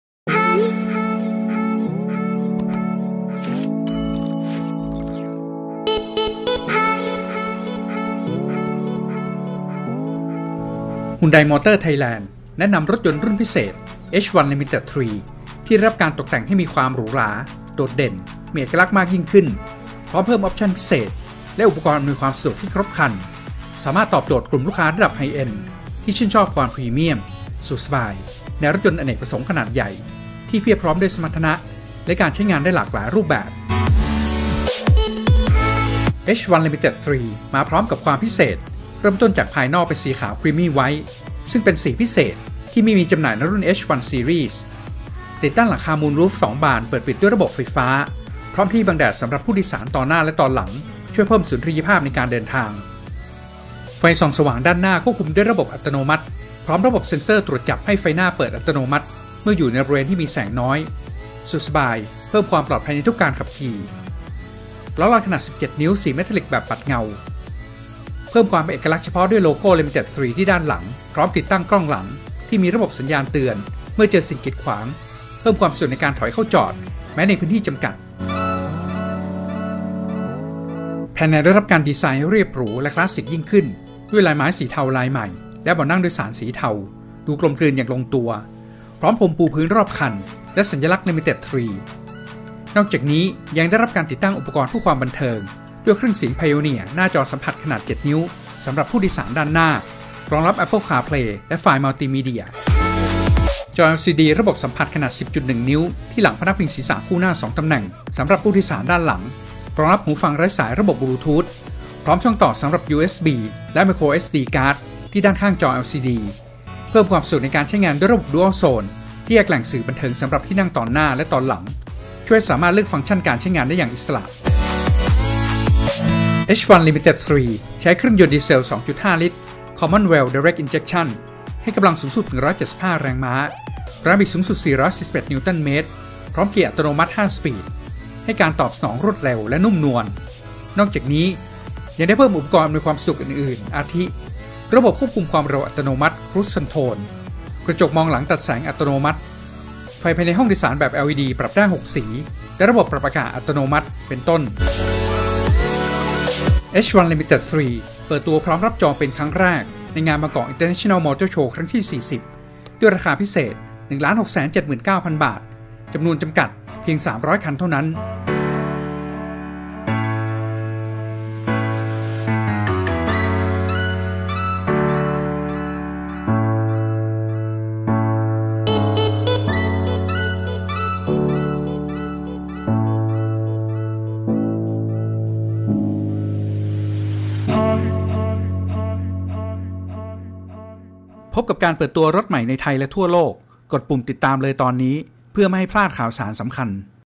เปิดตัว 2019 Hyundai H-1 Limited III รถตู้ รุ่นพิเศษ ราคา 1.679 ล้านบาท เพียง 300 คัน | CarDebuts
เปิดตัว 2019 Hyundai H-1 Limited III ฮุนได เอชวัน ลิมิตเต็ด 3 รถตู้โดยสารอเนกประสงค์ รุ่นพิเศษ ราคา 1.679 ล้านบาท เพียง 300 คันเท่านั้น

ฮุนได มอเตอร์  (ไทยแลนด์) แนะนำรถยนต์รุ่นพิเศษ Hyundai H-1 Limited III 2019 “เอช-วัน ลิมิเต็ด ทรี” ที่ได้รับการตกแต่งให้มีความหรูหรา โดดเด่น มีเอกลักษณ์มากยิ่งขึ้น พร้อมเพิ่มออฟชั่นพิเศษ และอุปกรณ์อำนวยความสะดวกที่ครบครัน สามารถตอบโจทย์กลุ่มลูกค้าระดับไฮเอนด์ ที่ชื่นชอบความพรีเมี่ยม สะดวกสบาย ในรถยนต์อเนกประสงค์ขนาดใหญ่ ที่เพียบพร้อมด้วยสมรรถนะ และการใช้งานได้หลากหลายรูปแบบ

Hyundai H-1 Limited III 2019 “เอช-วัน ลิมิเต็ด ทรี” มาพร้อมกับความพิเศษ เริ่มต้นจากภายนอกเป็นสีขาว ครีมมี่ ไวท์ ซึ่งเป็นสีพิเศษที่ไม่มีจำหน่ายในรุ่น เอช-วัน ซีรีส์ ติดตั้งหลังคามูนรูฟ 2 บาน เปิด - ปิด ด้วยระบบไฟฟ้า พร้อมที่บังแดดสำหรับผู้โดยสารตอนหน้าและตอนหลัง ช่วยเพิ่มสุนทรียภาพในการเดินทาง

ไฟส่องสว่างด้านหน้าควบคุมด้วยระบบอัตโนมัติ พร้อมระบบเซ็นเซอร์ตรวจจับให้ไฟหน้าเปิดอัตโนมัติ เมื่ออยู่ในบริเวณที่มีแสงน้อย สะดวกสบาย เพิ่มความปลอดภัยในทุกการขับขี่  ล้ออัลลอยขนาด 17 นิ้ว สีเมทาลิคแบบปัดเงา เพิ่มความเป็นเอกลักษณ์เฉพาะด้วยโลโก้ “Limited III” ที่ด้านหลัง พร้อมติดตั้งกล้องหลัง ที่มีระบบสัญญาณเตือนเมื่อเจอสิ่งกีดขวาง เพิ่มความสะดวกในการถอยเข้าจอด แม้ในพื้นที่จำกัด

ภายใน Hyundai H-1 Limited III 2019 ได้รับการดีไซน์ให้เรียบหรูและคลาสสิคยิ่งขึ้น ด้วยลายไม้สีเทาลายใหม่ และเบาะนั่งโดยสารสีเทา ดูกลมกลืนอย่างลงตัว พร้อมพรมปูพื้นรอบคัน และสัญลักษณ์ “Limited III”  นอกจากนี้ ยังได้รับการติดตั้งอุปกรณ์เพื่อความบันเทิง ด้วยเครื่องเสียงไพโอเนียร์ หน้าจอสัมผัสขนาด 7 นิ้ว สำหรับผู้โดยสารด้านหน้า รองรับ Apple Car Play และไฟล์มัลติมีเดีย

จอ LCD แบบสัมผัสขนาด 10.1 นิ้ว ที่หลังพนักพิงศีรษะคู่หน้า 2 ตำแหน่ง สำหรับผู้โดยสารด้านหลัง รองรับหูฟังไร้สายระบบบลูทูธ พร้อมช่องต่อสำหรับ USB และ Micro SD Card ที่ด้านข้างจอ LCD  เพิ่มความสะดวกในการใช้งานด้วยระบบ Dual Zone ที่แยกแหล่งสื่อบันเทิงสำหรับที่นั่งตอนหน้า และตอนหลัง ช่วยให้สามารถเลือกฟังก์ชั่นการใช้งานได้อย่างอิสระ

Hyundai H-1 Limited III 2019 “เอช-วัน ลิมิเต็ด ทรี” ใช้เครื่องยนต์ดีเซล 2.5 ลิตร คอมมอนเรล ไดเร็คท์อินเจ็คชั่น ให้กำลังสูงสุด 175 แรงม้า  แรงบิดสูงสุด 441 นิวตัน-เมตร พร้อมเกียร์อัตโนมัติ 5 สปีด ให้การตอบสนองรวดเร็วและนุ่มนวล นอกจากนี้ ยังได้เพิ่มอุปกรณ์อำนวยความสะดวกอื่นๆ อาทิ ระบบควบคุมความเร็วอัตโนมัติ (Cruise Control), กระจกมองหลังตัดแสงอัตโนมัติ, ไฟภายในห้องโดยสารแบบ LED ปรับได้ 6 สี และระบบปรับอากาศอัตโนมัติ เป็นต้น

Hyundai H-1 Limited III 2019 “เอช-วัน ลิมิเต็ด ทรี” เปิดตัวพร้อมรับจองเป็นครั้งแรกในงาน “บางกอกอินเตอร์เนชั่นแนล มอเตอร์โชว์ ครั้งที่ 40” ด้วยราคาพิเศษ 1,679,000 บาท จำนวนจำกัด เพียง 300 คันเท่านั้น ผู้ที่สนใจสามารถเยี่ยมชมและทดลองขับ “เอช-วัน ลิมิเต็ด ทรี” ได้ที่บู้ธรถยนต์ฮุนไดในงานฯ ณ อิมแพ็ค เมืองทองธานี ระหว่างวันที่ 27 มี.ค. – 7 เม.ย. 2562 และที่โชว์รูมรถยนต์ฮุนไดทั่วประเทศ ติดตามช่องของเราทาง Facebook ได้ที่